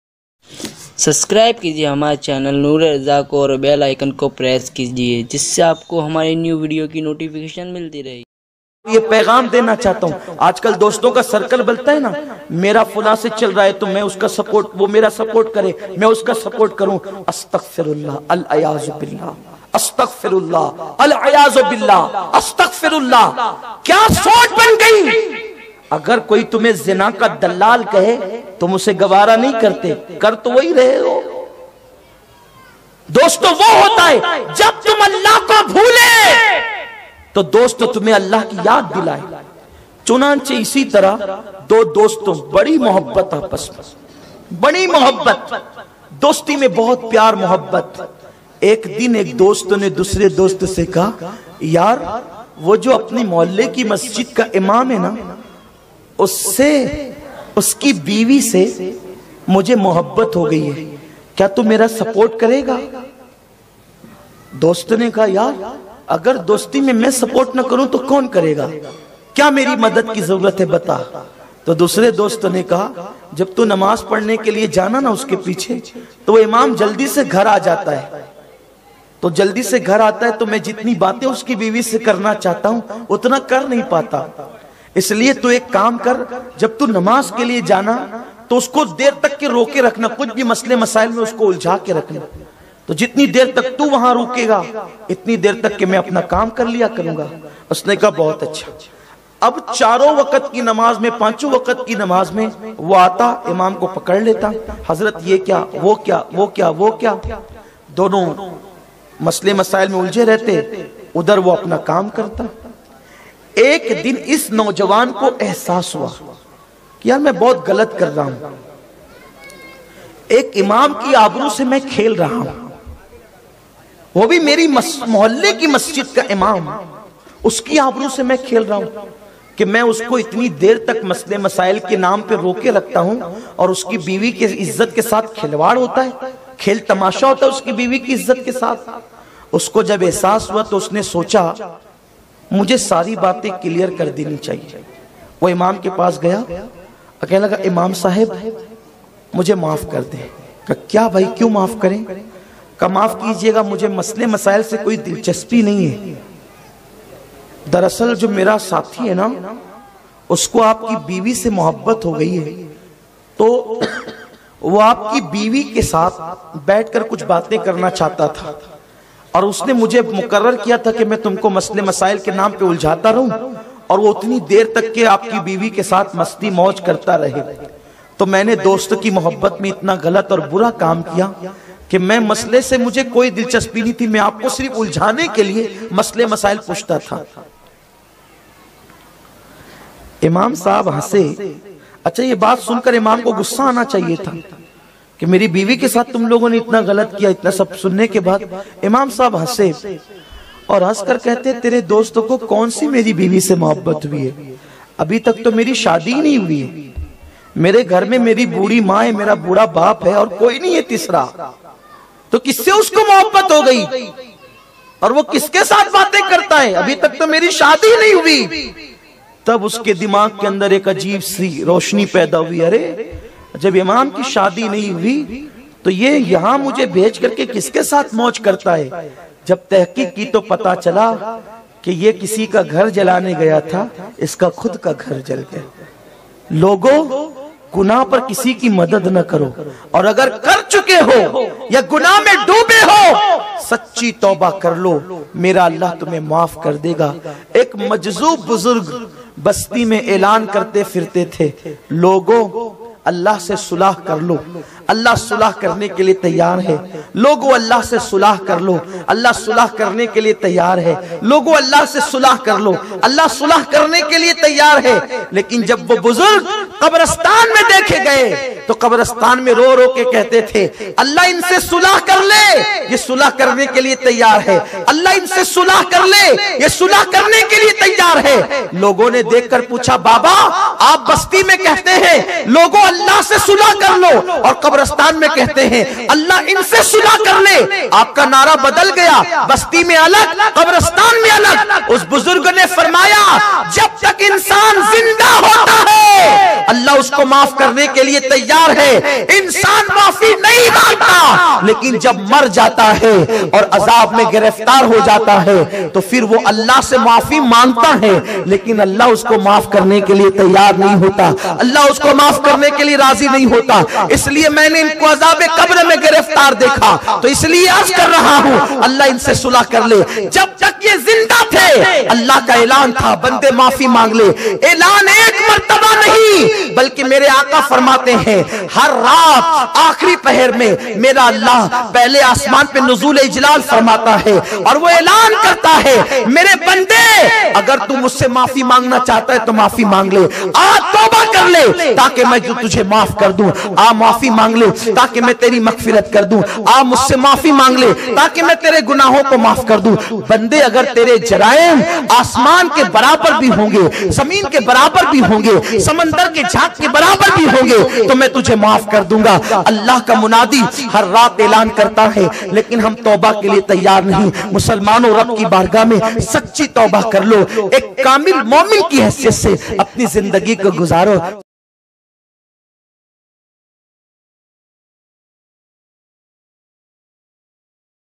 Dosti Mein Is Baat Ka Hamesha Dhyan Rakhna | Sayyed Aminul Qadri Bayan
New Bayan - Sayyed Aminul Qadri Dosti mein is baat ka hamesha Dhyan rakhna

Full Bayan -